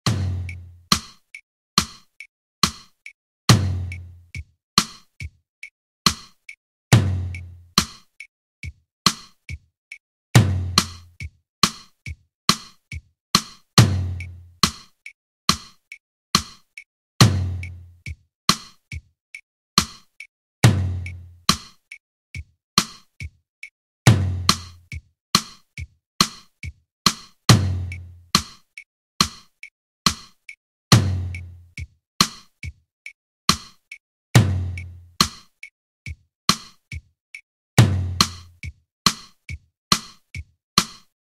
Tutorial: Tommy Igoe's Syncopated Rhythm Drumming Exercise part 2 - how to set up Bounce Metronome
This was posted as an answer to a user who asked how to set up Bounce Metronome for exercises such as this one from Tommy Igoe's "Great Hands for a Lifetime drumming" instruction video. For details of how you do this in Bounce Metronome, for this one and other exercises of this type, follow this